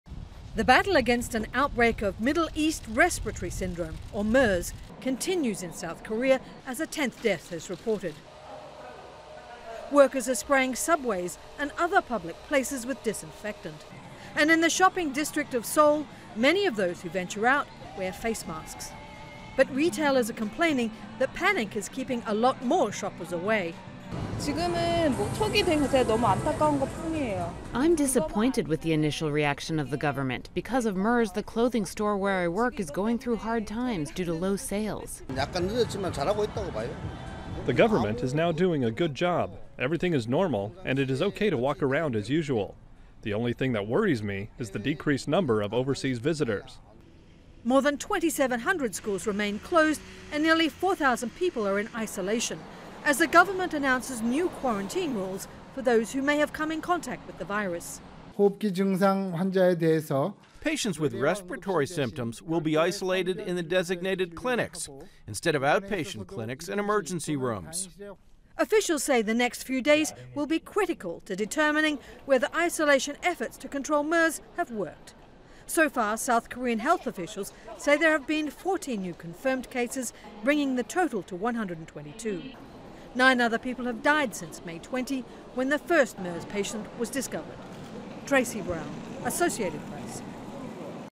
South Korea Reports 10th Death from MERS Virus
South Korean officials believe the MERS outbreak has peaked, even as they report a 10th person has died from virus. The outbreak of Middle East respiratory syndrome has caused panic in the country. (June 11)